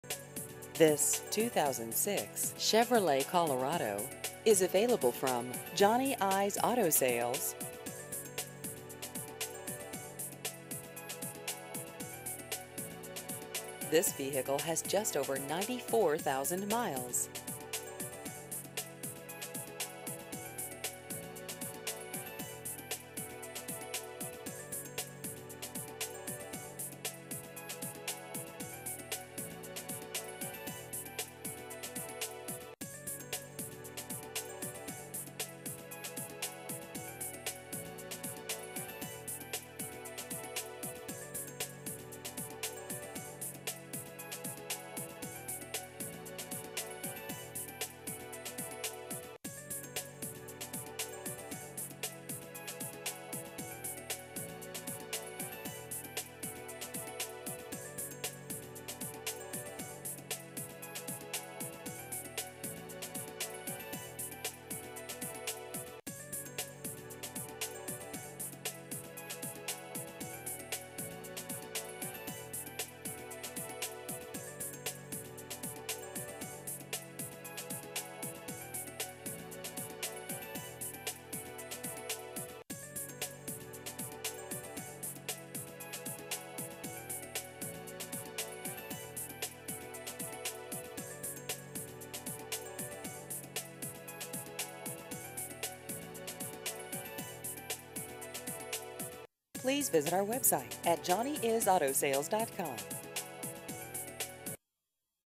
2006 Chevrolet Colorado Used Cars Lawrence KS
This 2006 Chevrolet Colorado is available from Johnny I's Auto Sales.